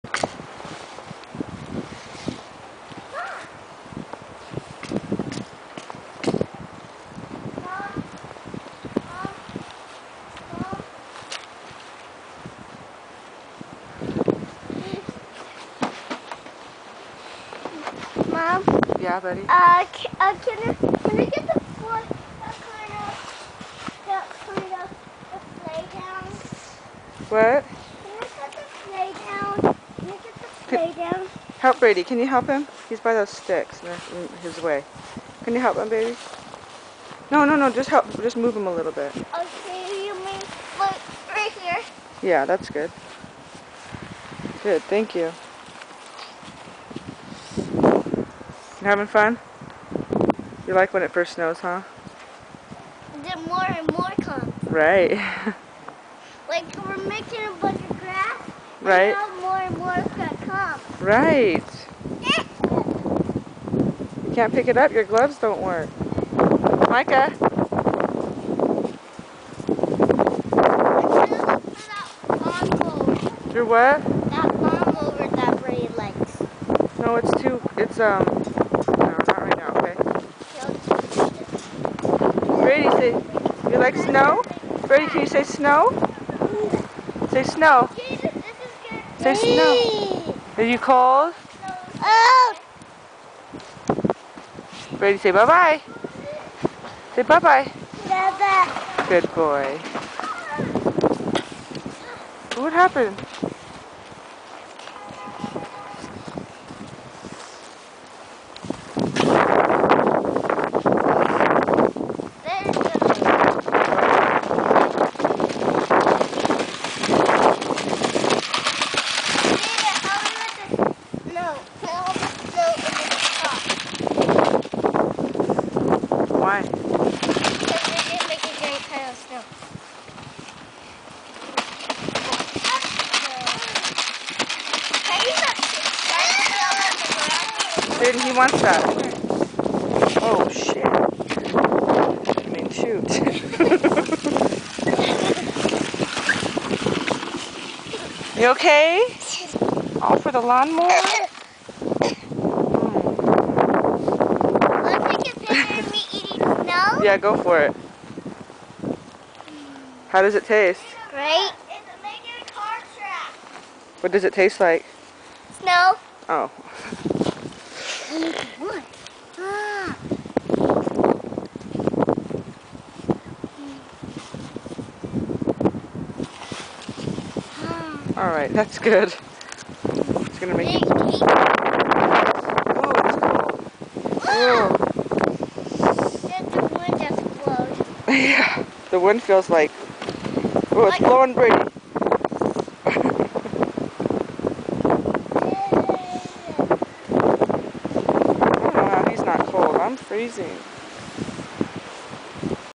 Brady out in the snow for first time
He LOVED it.